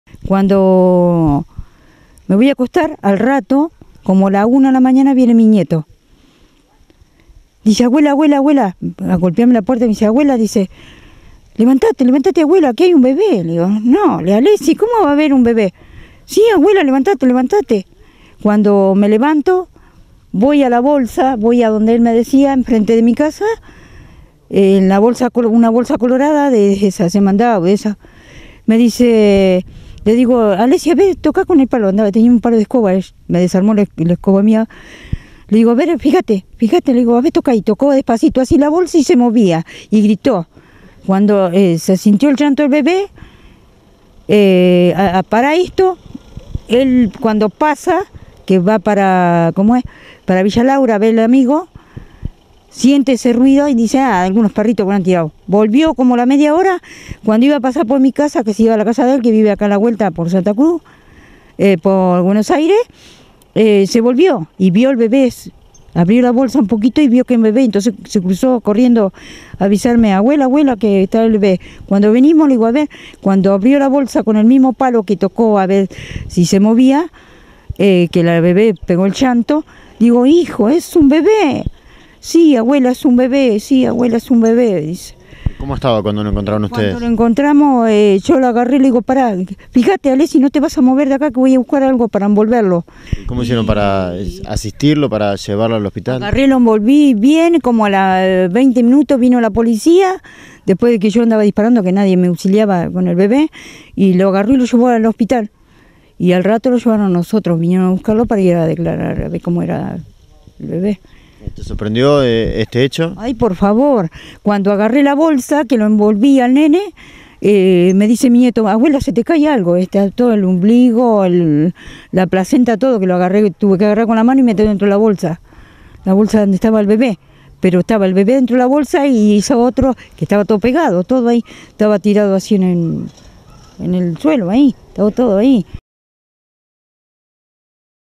APARECIÓ UNA BEBA ABANDONADA EN PUNTA ALTA
Habló la abuela del joven que la encontró